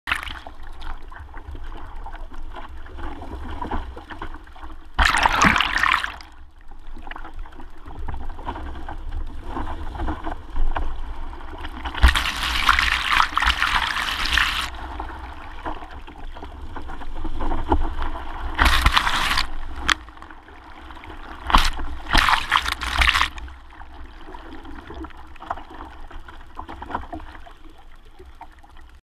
Some fun off the Big Cumbrae
Paddle sailing a fully loaded expedition kayak through the Farland Point tide race.